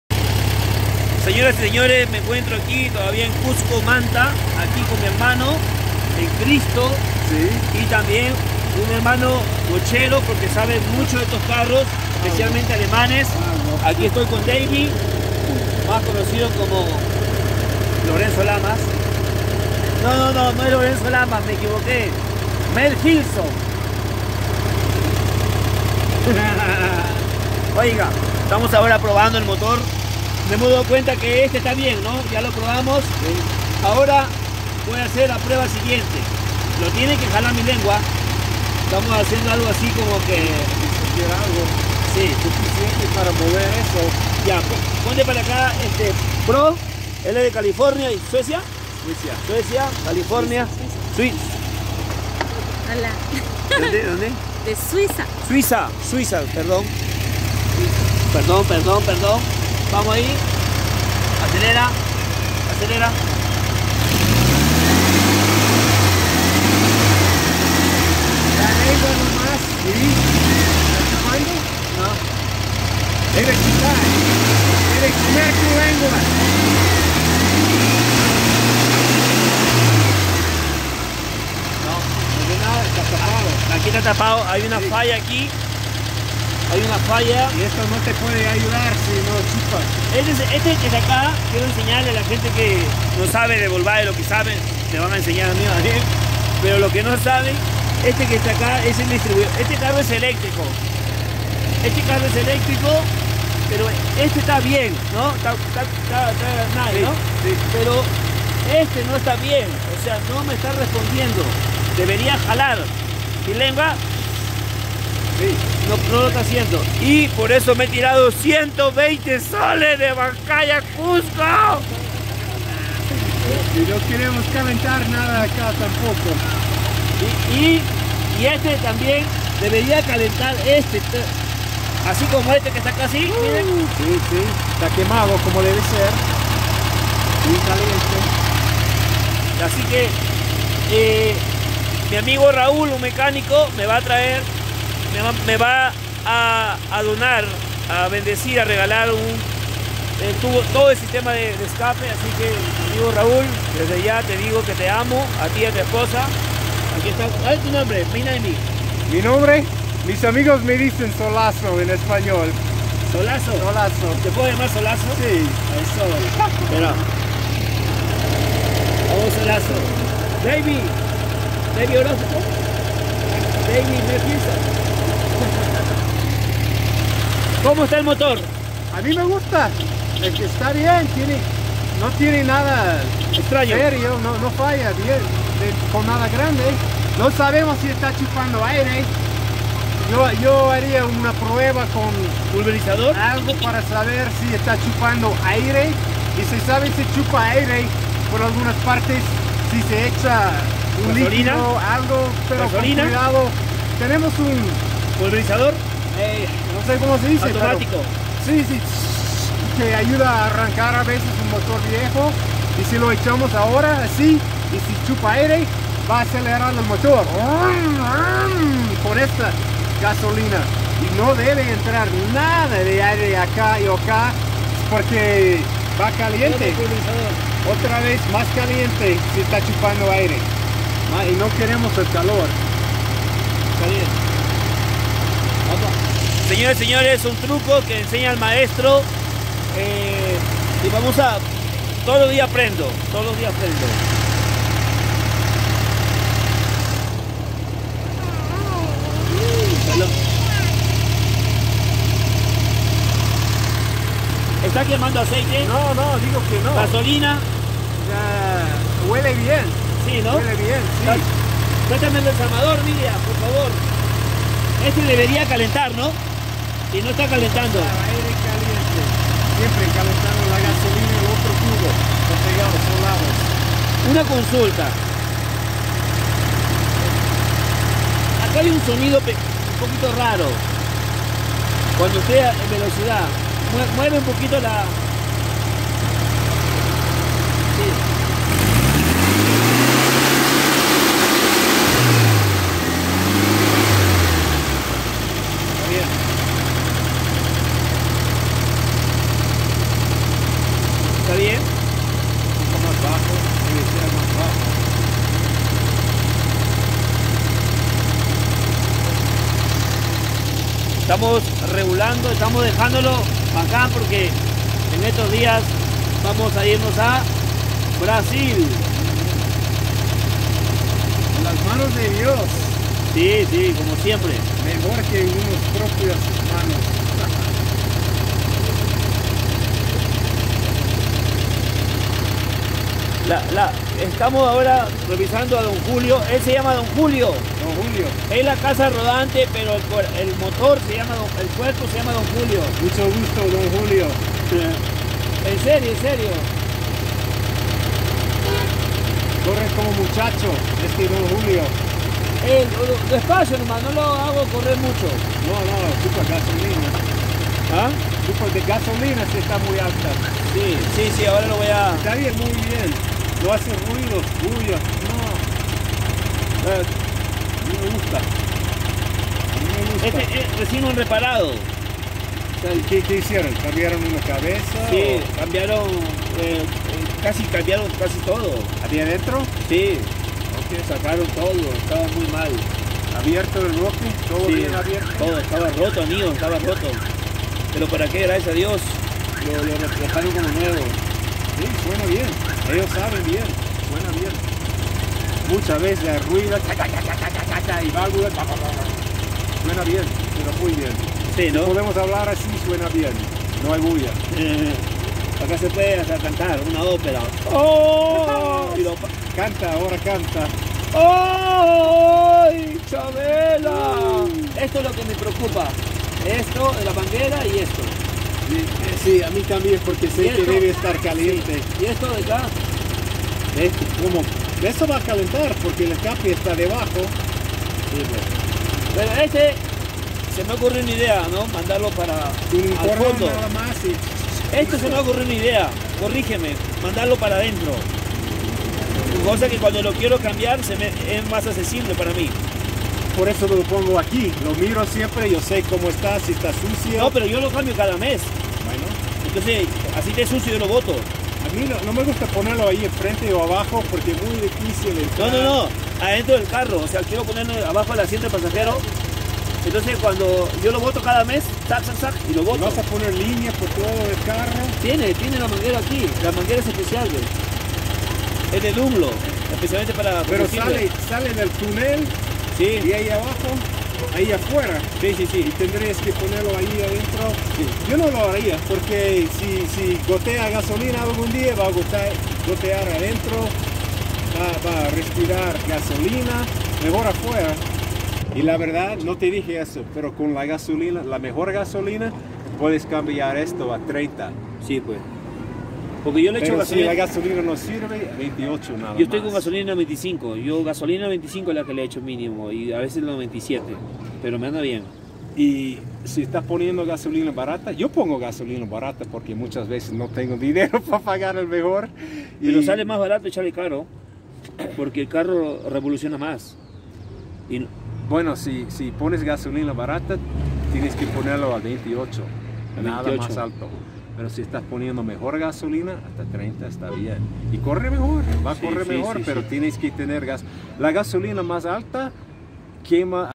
Un Americano me ESCUELEA EN VOCHO
Que gran hombre conocimos en Cusco el se llama Rex, Dios siempre me manda gente de mil maravillosa...